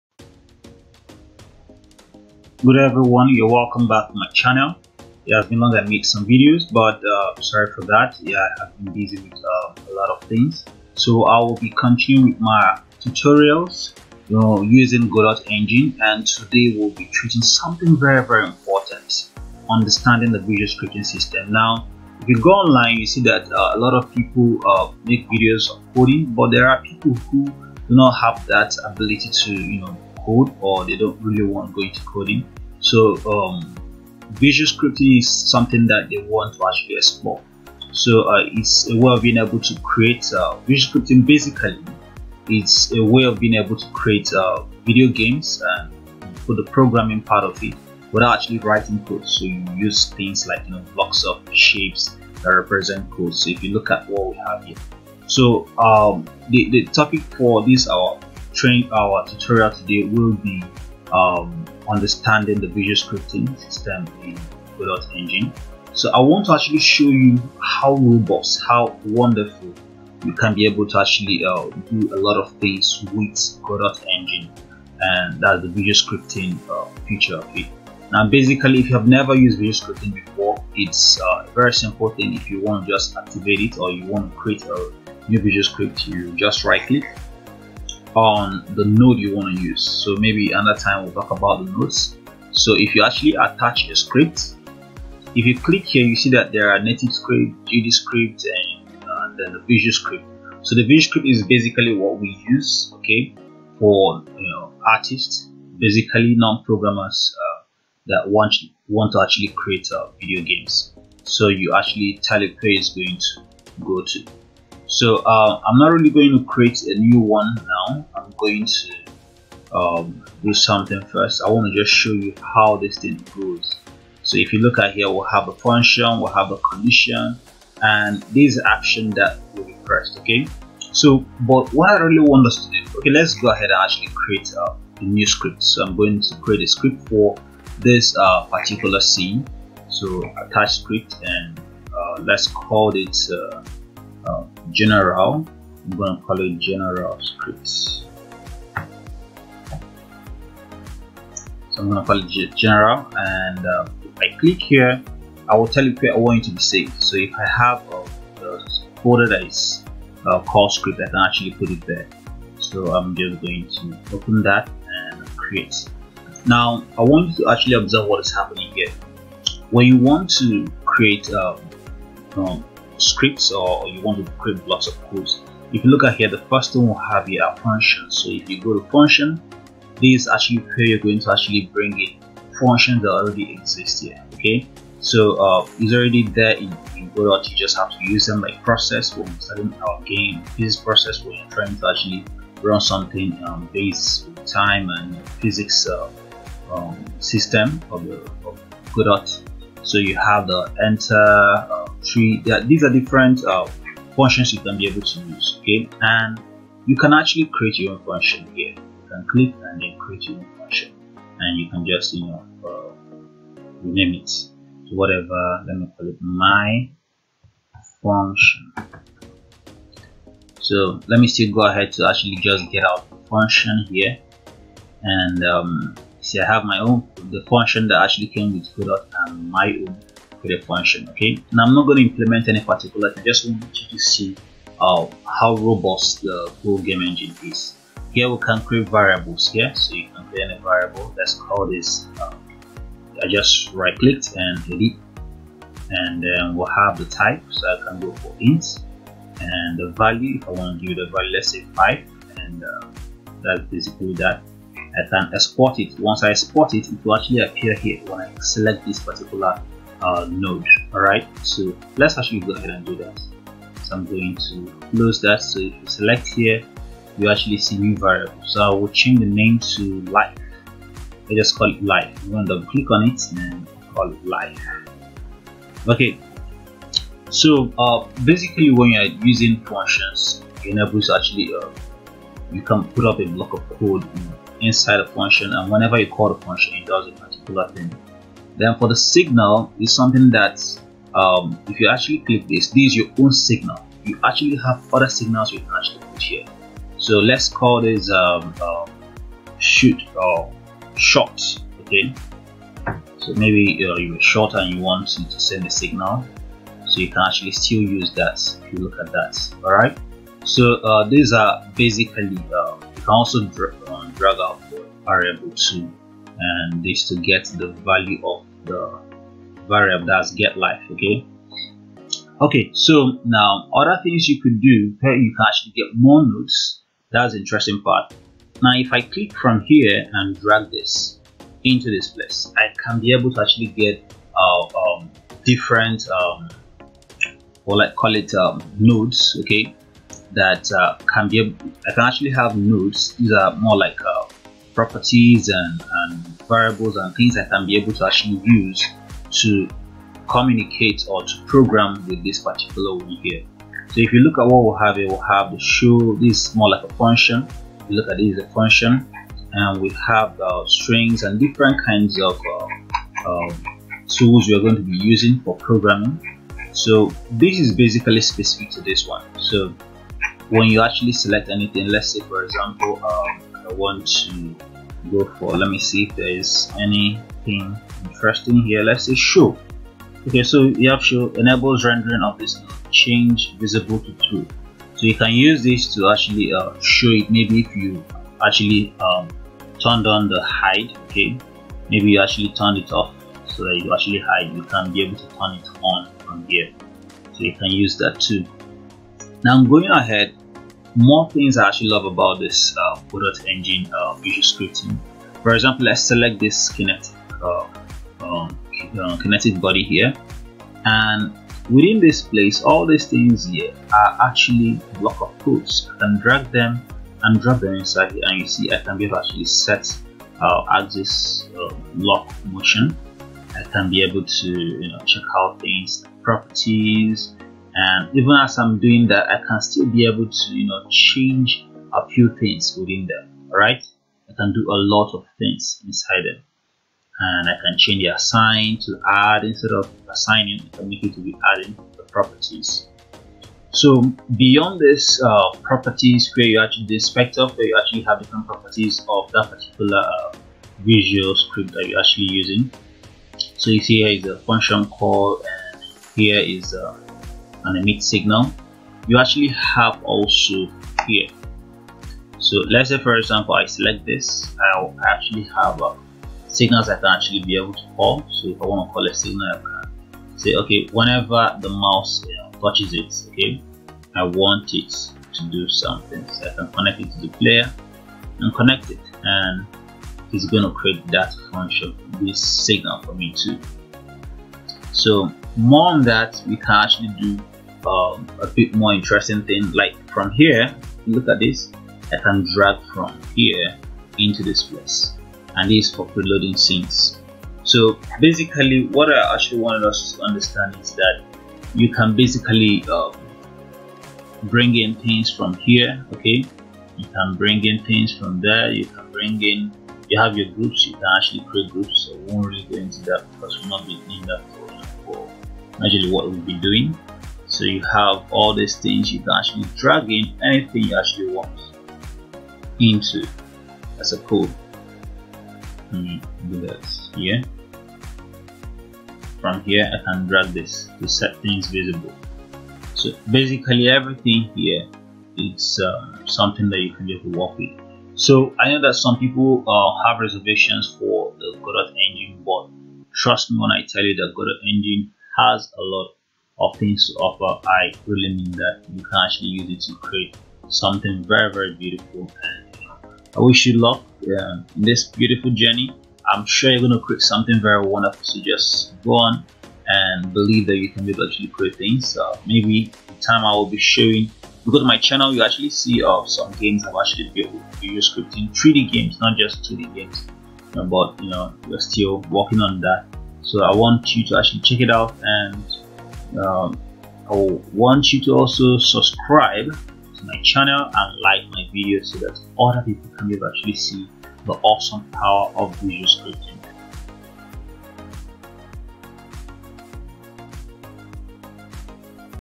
Understanding the godot visual scripting part 1
The visual scripting feature in godot game engine is a very good option for 3d artists and graphics designers who wish to create simple games without learning how to code. In this video I tried to demonstrate how you can easily create block of visual scripts from different parts of the godot interface.

The visual scripting system can enable an artist to prototype games and create a simple game for both entertainment or educational purpose.